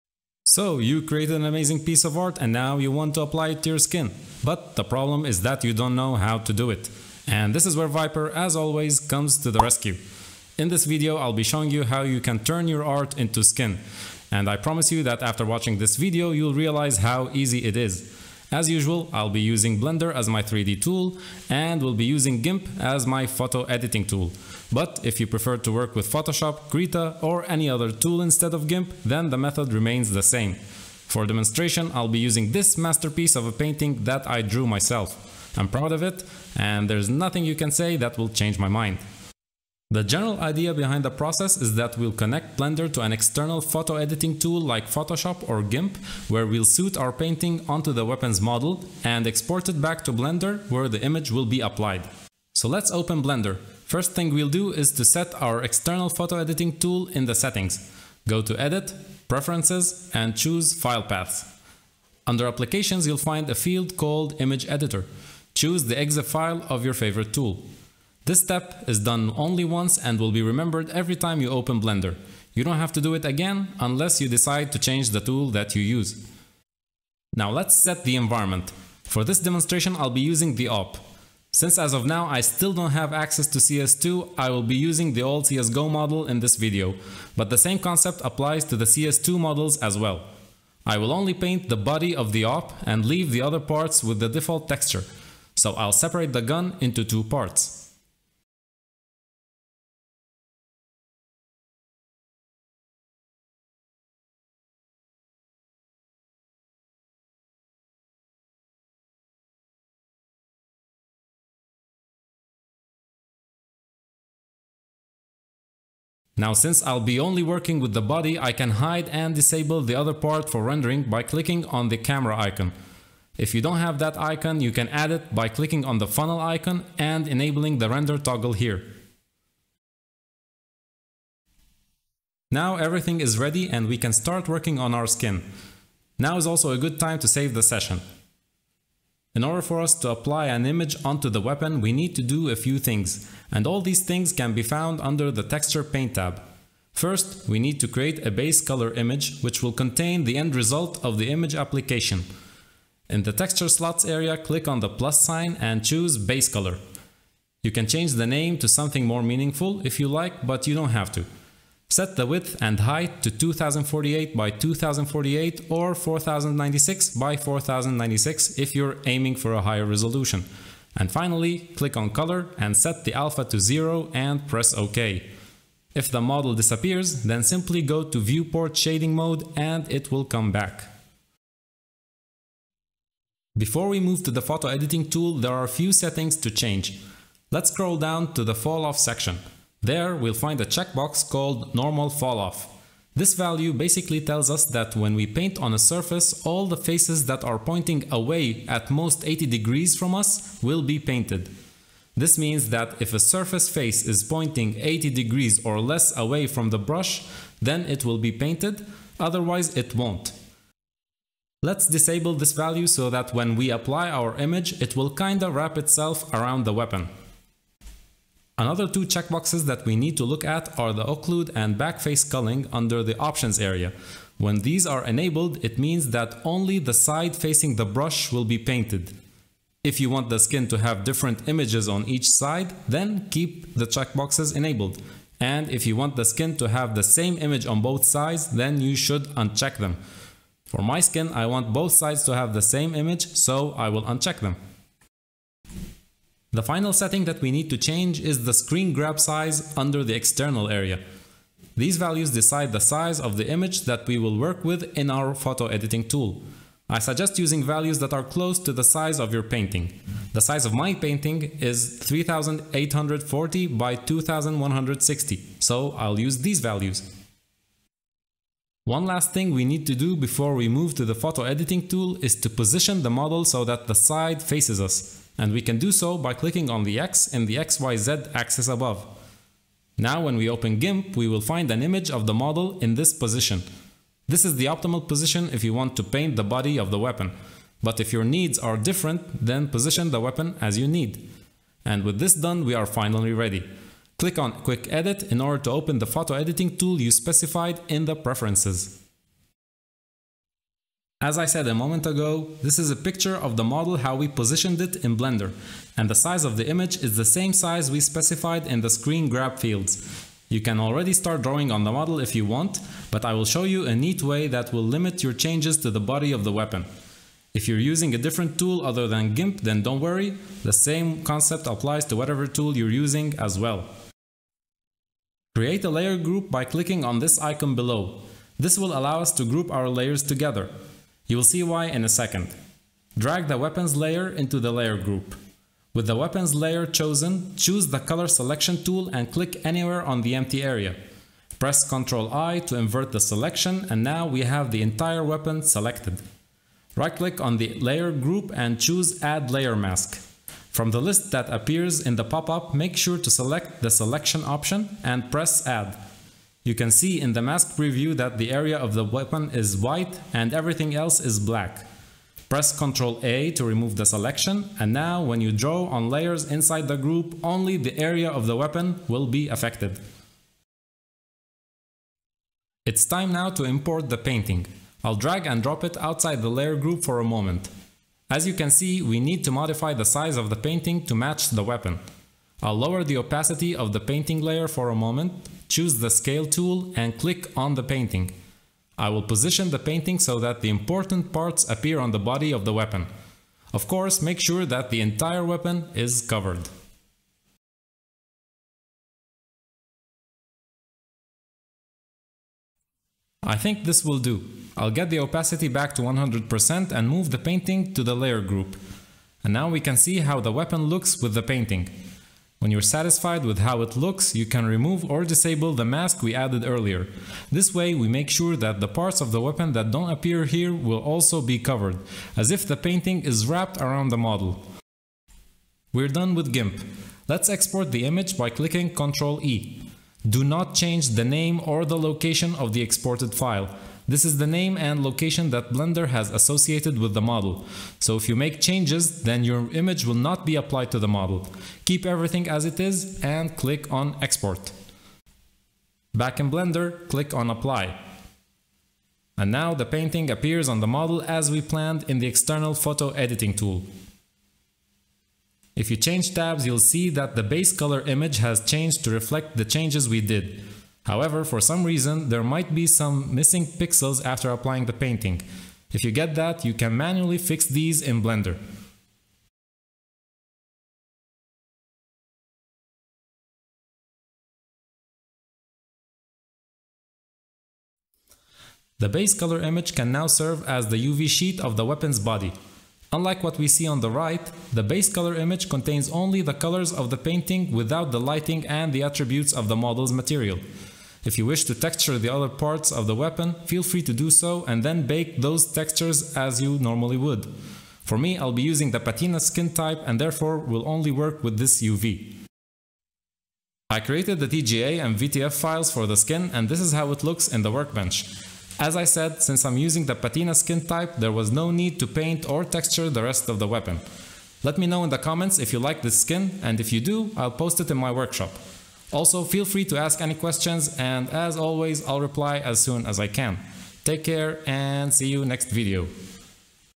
Turn your art into CS Skins using Blender
1st image in the video was downloaded from freepik.com

Follow these simple steps to turn your masterpiece of a painting or drawing into a winning CS skin!
In this video I use Blender as my 3D tool and GIMP as my photo editing tool, however you can use other similar tools like Photoshop or krita.